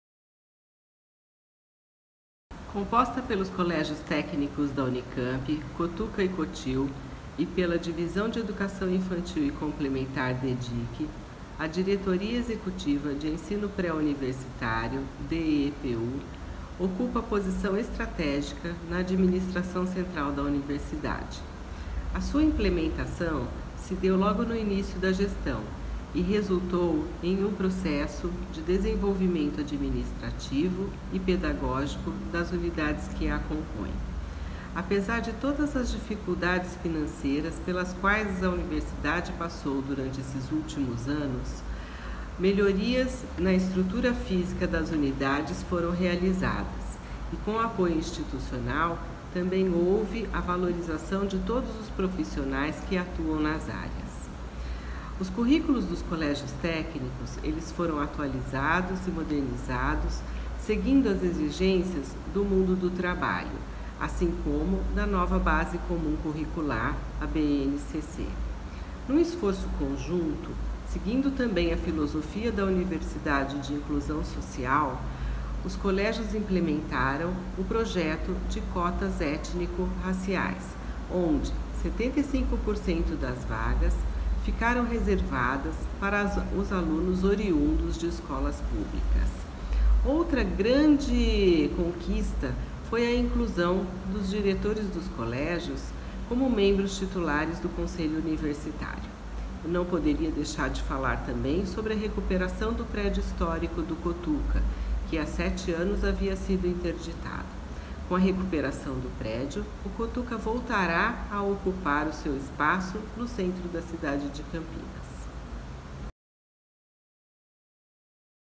Teresa Meloni Rosa (DEEPU) - Balanço de Gestão 2017-2021
A Diretoria Executiva de Ensino Pré-Universitário (DEEPU) da Unicamp foi criada nesta gestão e é responsável pela administração dos Colégios Técnicos de Campinas (Cotuca) e Limeira (Cotil) e pela Divisão de Ensino Infantil e Complementar (DEdIC). Diretora Executiva da unidade, Teresa Celina Meloni Rosa comenta sobre as melhorias realizadas na estrutura física das unidades, com destaque para a revitalização do prédio histórico do Cotuca, sobre a valorização dos profissionais ligados ao órgão e a atualização dos currículos dos colégios técnicos. Ela também lembra da implementação das cotas étnico-raciais nos processos seletivos das unidades, que garante 75% das vagas a alunos da rede pública de ensino.